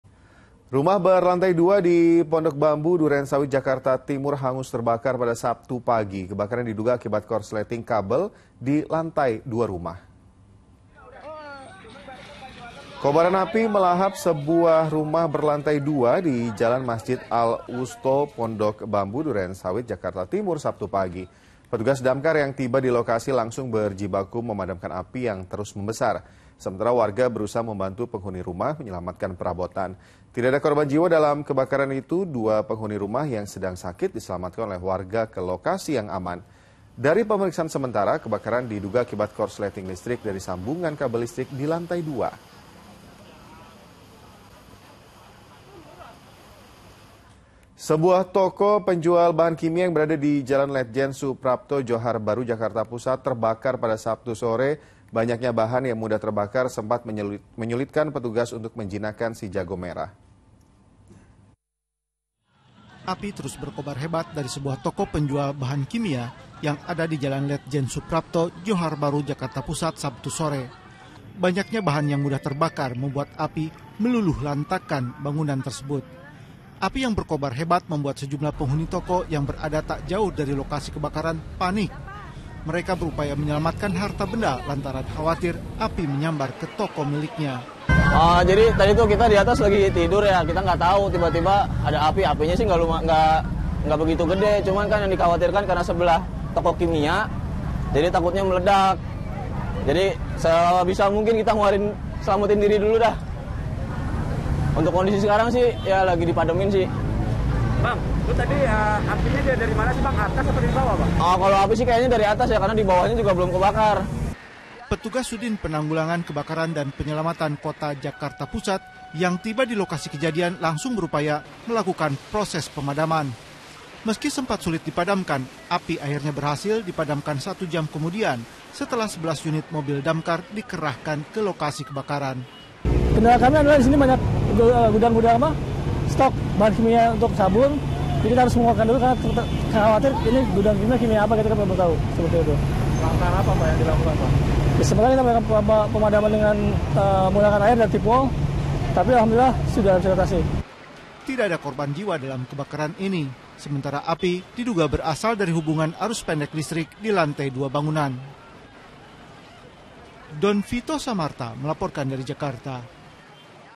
Akibat Korsleting Kabel, Rumah Dua Lantai di Duren Sawit Kebakaran | Liputan 6
Rumah berlantai dua di Pondok Bambu, Duren Sawit, Jakarta Timur, hangus terbakar, Sabtu (28/08) pagi. Kebakaran diduga akibat korsleting  kabel di lantai dua rumah.